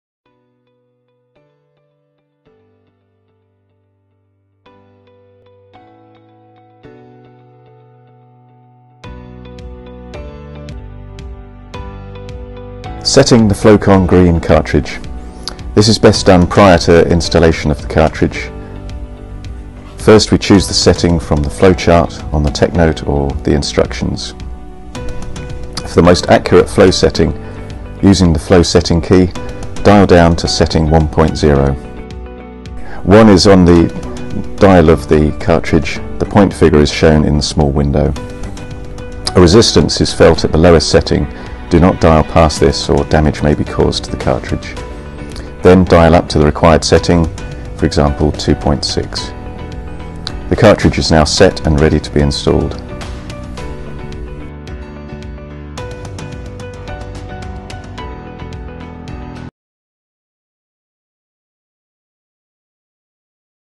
Setting the Flowcon GREEN PICV Cartridge - ATP
ATP demonstrate how easy it is to set the Flowcon GREEN PICV flow cartridge.
The removable cartridge allows them to be set away from the valve if required.
It also allows for easy maintenance; the cartridge can be removed, washed clean under a tap & returned into service. It can also be swapped for a different flow range - all without having to remove the valve body from the pipework.